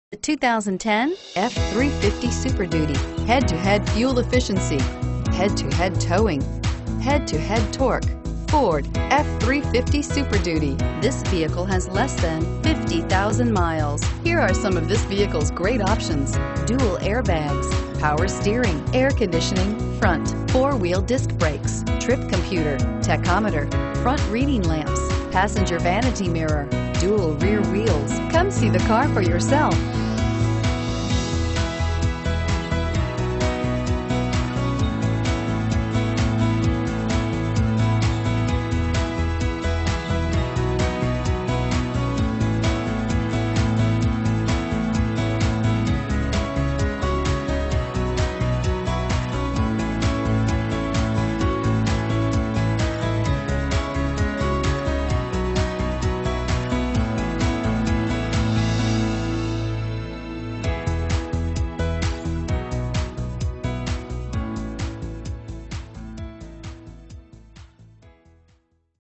2010 Ford F-350SD Douglas WY RP1163A
2010 Ford F-350SD

120 S. Riverbend Dr.

Pick up this White 2010 Ford F-350SD, available today at Greiner Chrysler Dodge Ram Jeep Ford. Featuring Automatic transmission, it has 45426 miles. This could be the one you've been searching for.